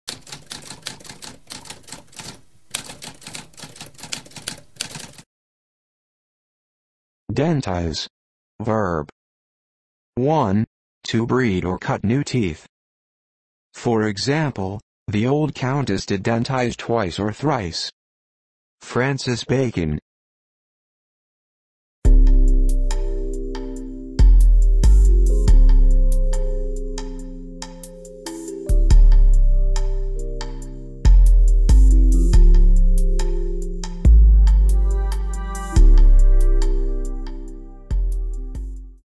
What does dentize mean?
A spoken definition of dentize.

Intro Sound:
Typewriter - Tamskp